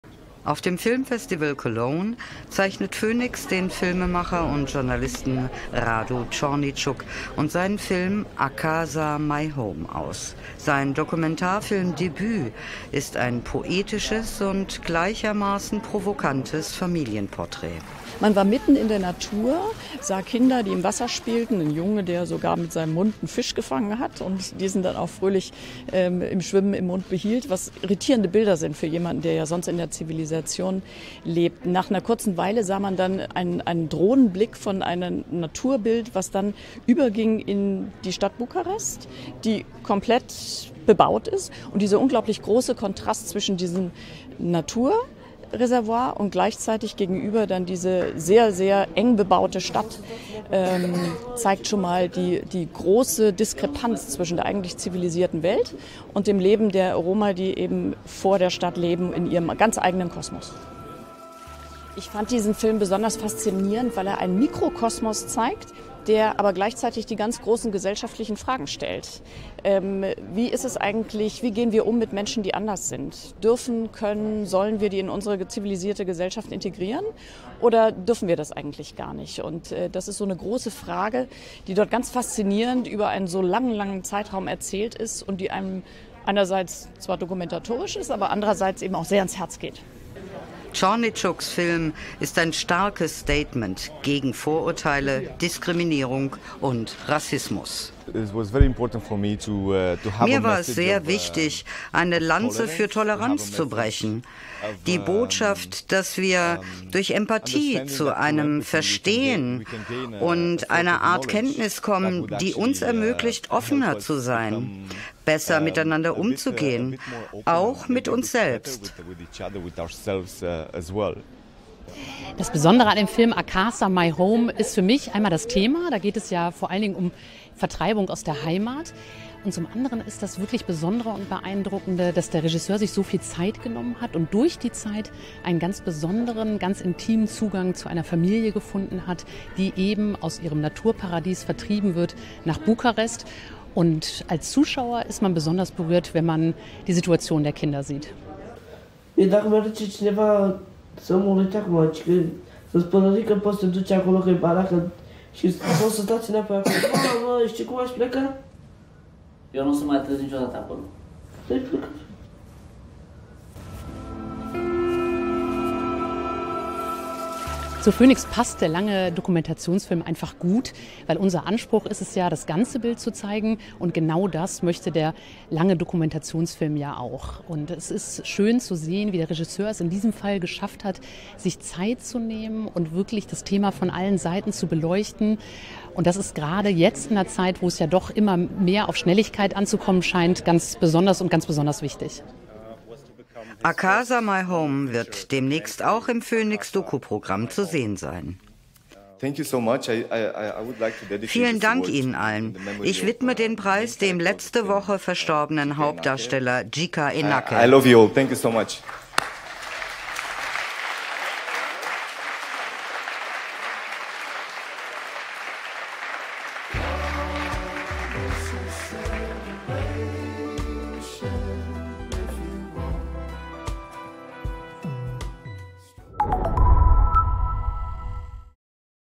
phoenixpreis für Radu Ciorniciuc und seinen Film „ACASA - MY HOME“
Der rumänische Journalist und Dokumentarfilmer Radu Ciorniciuc wird im Rahmen des Film Festival Cologne mit dem phoenixpreis ausgezeichnet. Damit würdigt der Dokumentations- und Ereigniskanal sein Erstlingswerk: „ACASA - MY HOME“. Er hat hiermit einen herausragenden Dokumentarfilm mit einem hervorragenden Auge für menschliche Geschichten von gesellschaftlicher Bedeutung geschaffen. Auch sein soziales Engagement, das er in diesem eindringlichen Film mit Gefühl und Tiefgang umsetzt, hat überzeugt.

Der Film ist Ergebnis einer vierjährigen Langzeitbeobachtung einer elf-köpfigen Familie, die am Rand der Gesellschaft in Sichtweite der Stadt Bukarest lebt. Hier, in einem abgeschieden Flussdelta, aber in Einklang mit der Natur wohnt das Paar mit ihren neun Kindern, bevor sie zwangsweise umgesiedelt und auseinander gerissen werden. Radu Ciorniciuc hat das Leben und den sozialen Bruch dieser besonderen Großfamilie nicht nur porträtiert, er hat sie auch umfassend betreut, und ihr geholfen, wieder zusammen zu finden.